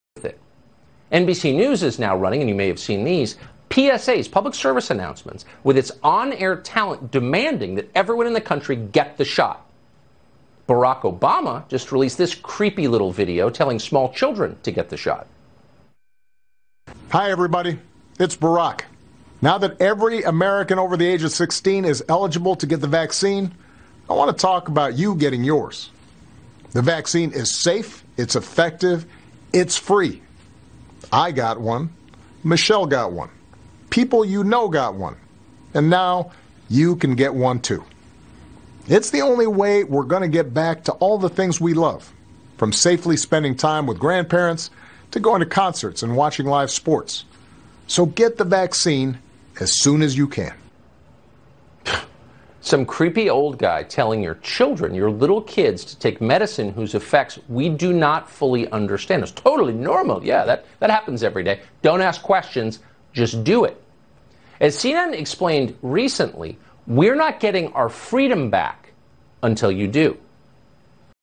Tucker Carlson Railed Against Barack Obama 'Creepy Old Guy' for doing a PSA to get vaccinated
Tucker Carlson reacts to Former President Barack Obama doing a PSA calling on more Americans to get vaccinated after teenagers over 16 were approved to get the shot.